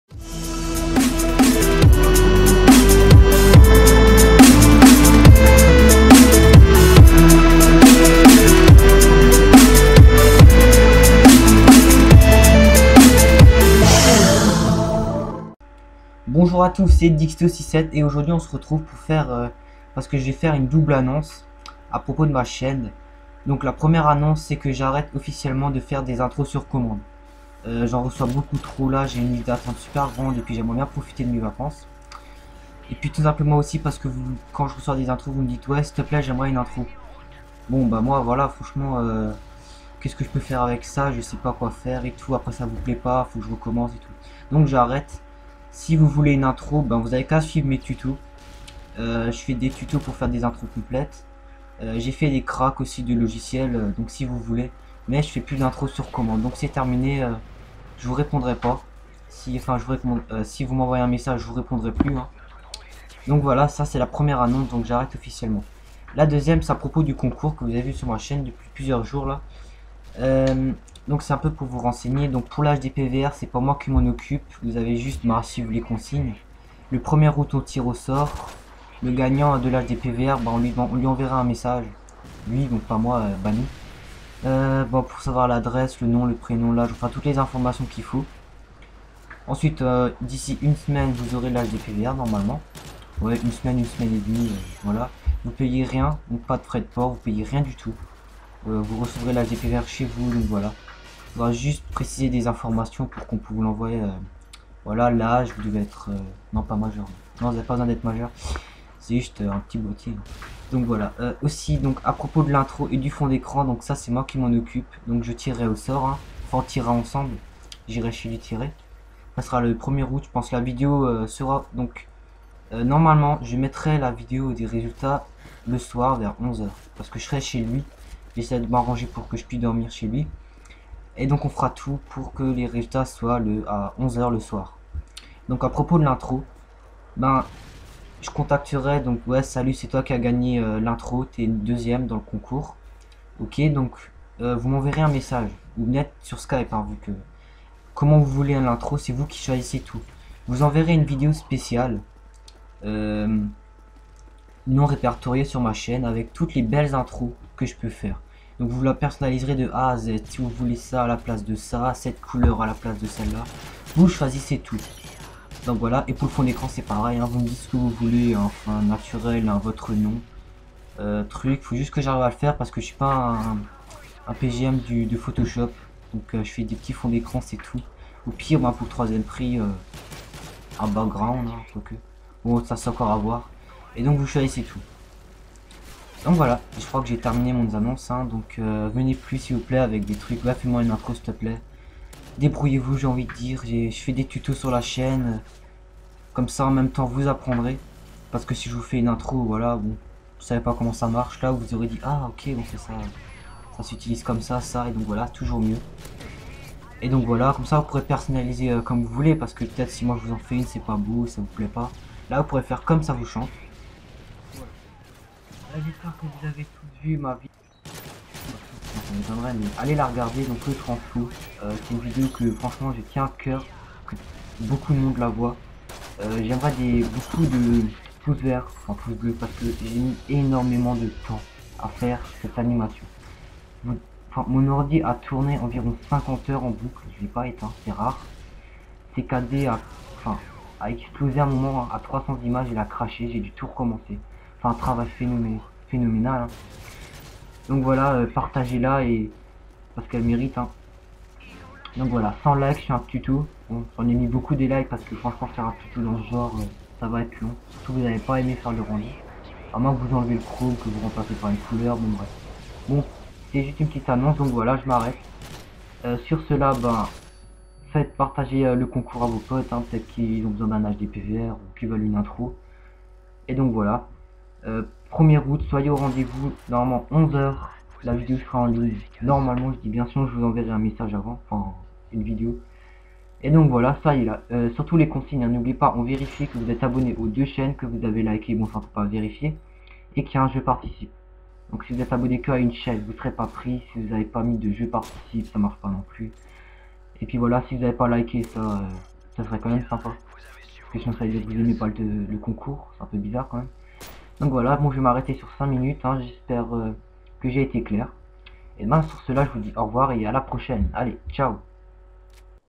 Annonces Importantes ( Intro + Concours )
Ben écoutez attentivement ;)

Deux mots sur le gameplay : Nul, wtf
A la fin : 2 magnifique de trickshot de moi que je trouve très bizarre car dans aucun des deux le sniper s'aligne avec le gars ^^